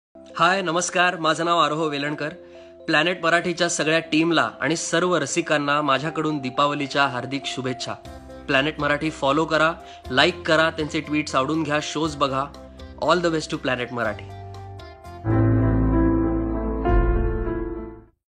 Aroh Welankar wishing all viewers a Happy Diwali 2017 | PlanetMarathi
रेगे चित्रपटा मधून रसिक मनाचा ठाव घेणारा तरुण अभिनेता आरोह वेलणकर ने दिल्या दीपावलीच्या शुभेच्छा...
Diwali is round the corner and celebrating this occassion , many friends and artistes have come out with their Bestwishes to planetmarathi followers !